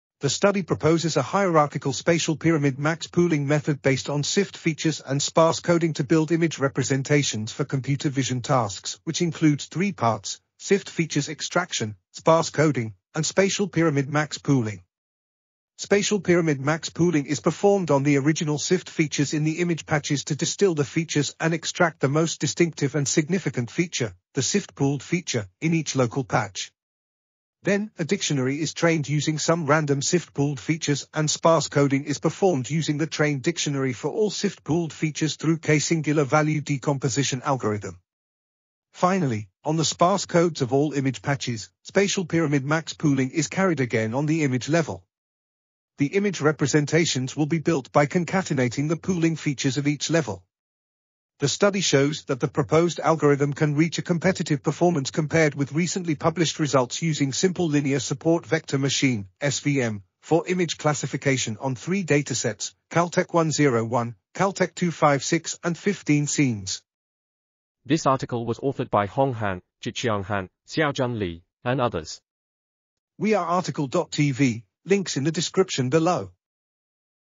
Hierarchical spatial pyramid max pooling based on SIFT features and sparse coding for... | RTCL.TV
### Keywords ###

### Article Attribution ###
Title: Hierarchical spatial pyramid max pooling based on SIFT features and sparse coding for image classification
Authors: Hong Han, Qiqiang Han, Xiaojun Li ,and Jianyin Gu
Publisher: Wiley
DOI: 10.1049/iet-cvi.2012.0145

### Video Timestamps ###
0:00:00 - Summary
0:01:07 - Title
0:01:12 - Tail
0:01:17 - End